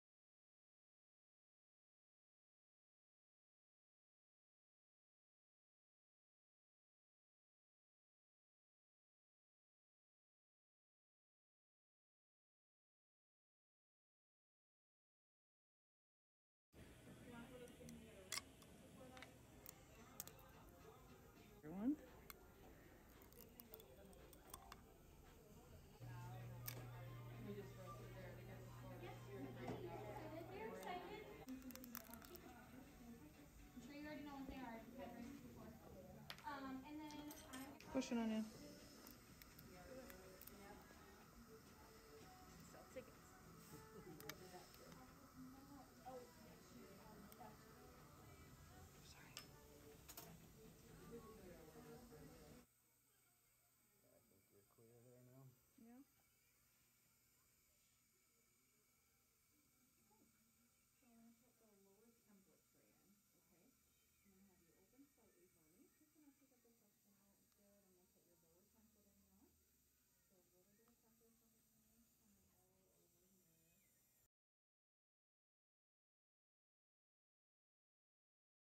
Braces On Demand - Monolithic vs. Digital Indirect Bonding With Trays
See the difference between digital indirect bonding with trays vs. Monolithic.  Remove sections for an entire case in half the time it takes to remove a single tray!  Learn more about how Braces On Demand is simplifying IDB